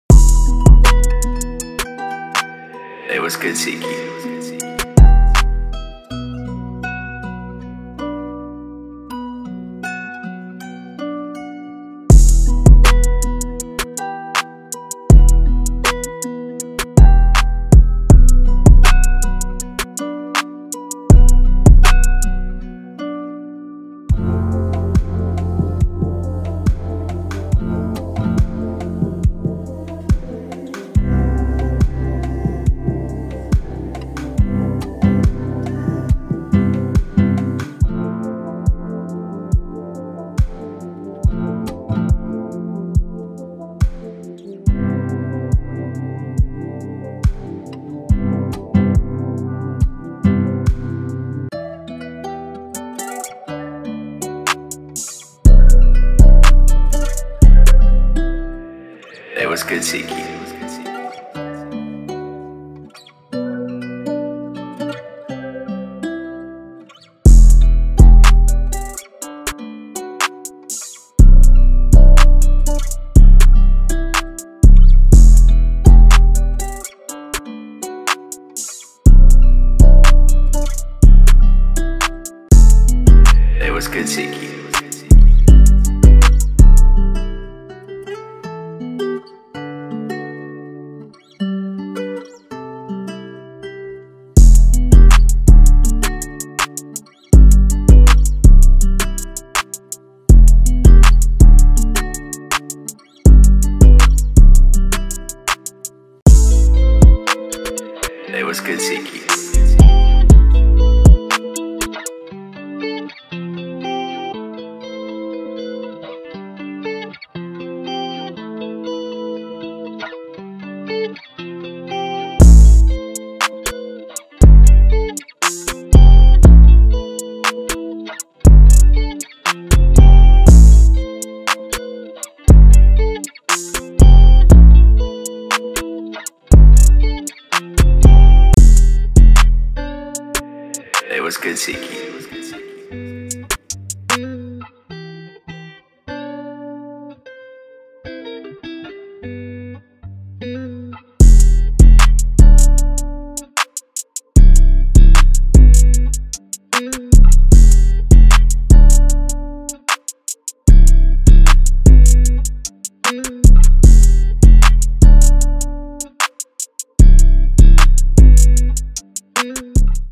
"RIPTIDE" Multi-Kit - Live Guitar Chord Toolkit, R&B Oneshots, and more
"Riptide" is a collection of live recorded samples, real analog one shot sounds, live guitar tool-kit featuring sounds and phrases (nylon, electric, bass), portal, shaperbox & mixer presets by @Loopsbyciki

The samples in this pack have been recorded at high-quality, without the use of VSTs for a more authentic sound.

This kit contains:

SAMPLES:

- 25 Samples in .WAV format

- 25 Root note MIDIs

- No BeatStars sales splits required

ONE SHOT KIT:

- 150 Analog Synth sounds (Moog, Korg, Kawaii k4, Yamaha)

- Processed through FX pedals & Tape

- Textures, Synths, Pads, Runs, Basses, Bells, Pads

PRESETS/BANKS:

- 10 Shaperbox2 Presets

- 10 Shaperbox3 Presets

- 30 Portal Presets

- Live Guitar FX mixing chains

- Electric Guitar AMP Presets

GUITAR TOOL KIT:

-150+ Guitar Chord Shots (Live Electric and Nylon)

- 25+ Live Bass guitar shots (Open, Palm Muted)

- 20 Live Guitar Licks/Phrases

- Live Guitar Textures (Slides, Strums, Slaps)

🎥 vfx by @ripple.music_